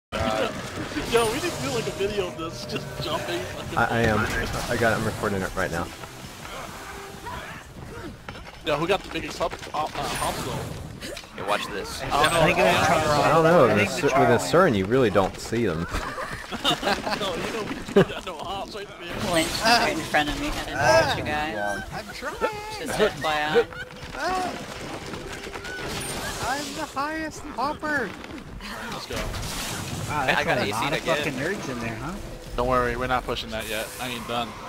Jump jump jump
Bodyblocking siege while preparing to take tower